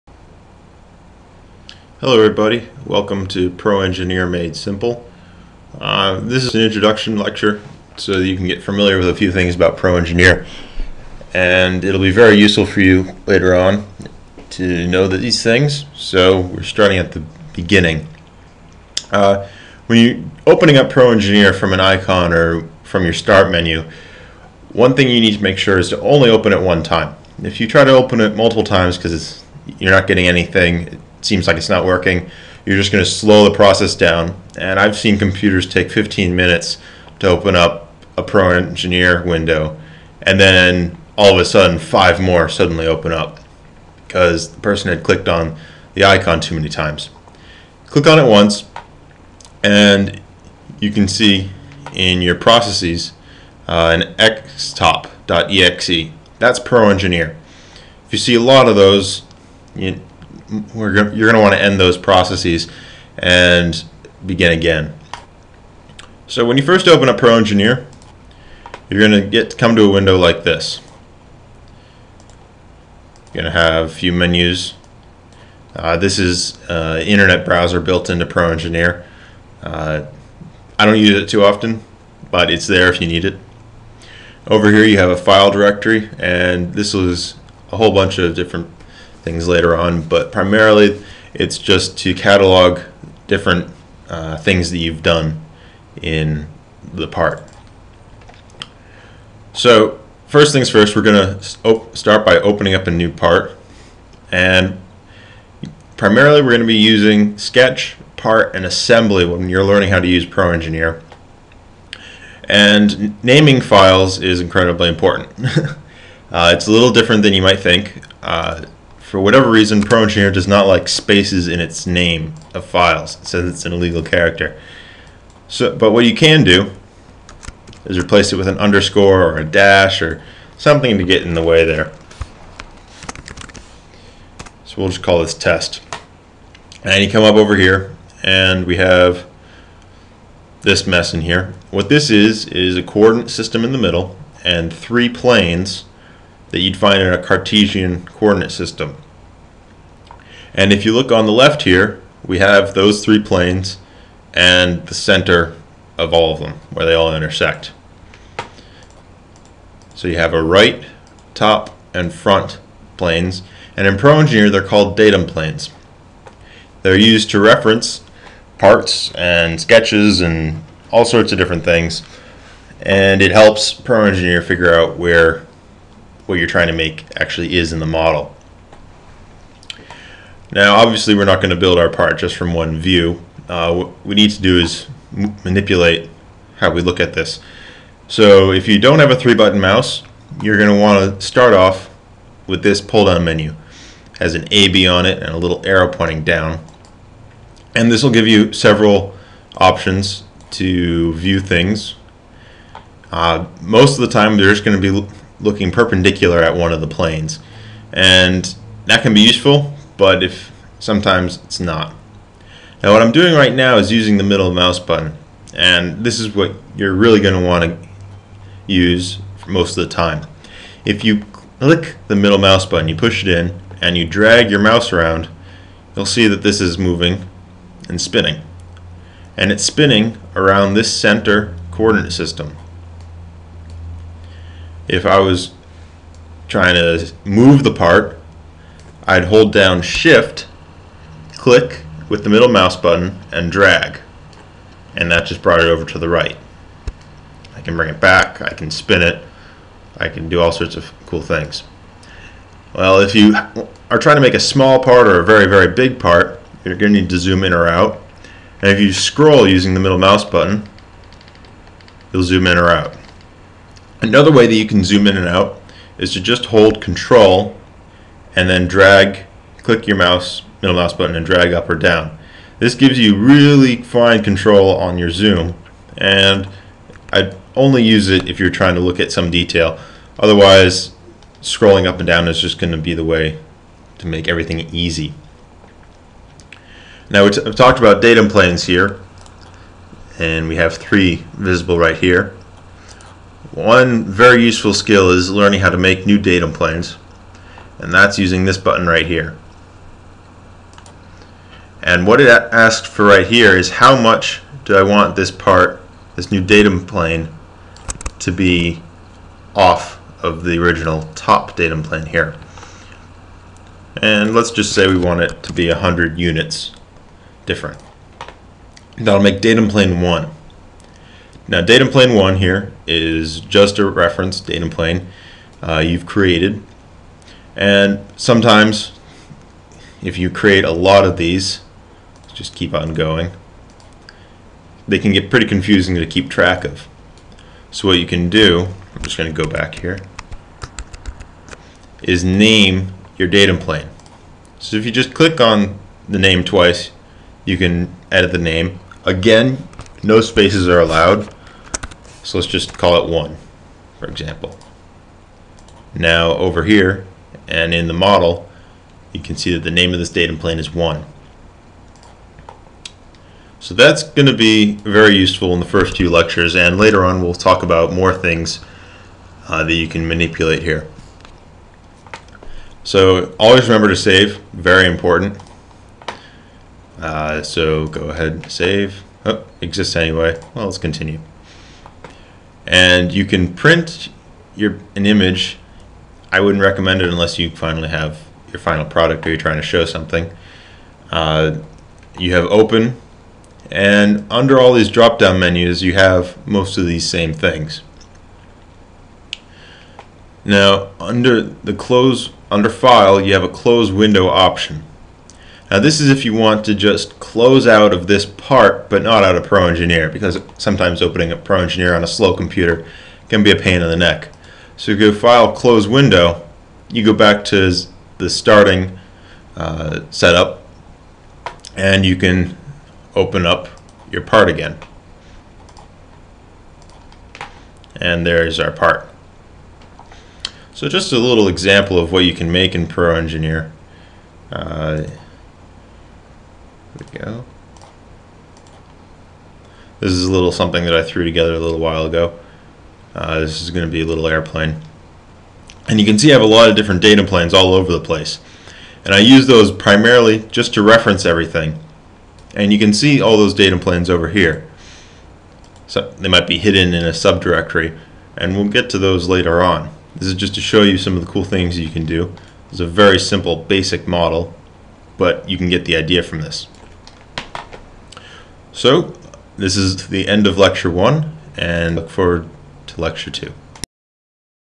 Pro/Engineer Made Simple - Part 1 - Introduction to Pro/Engineer
Part one of my tutorial series Pro/Engineer Made Simple.

Includes:
-Rules about naming files
-Rotating, zooming and moving your view
-Datum Planes

*This is meant to be a supplementary tool in your process of learning to use Pro/Engineer.

**There is no substitute for playing around with the software and learning by trial and error.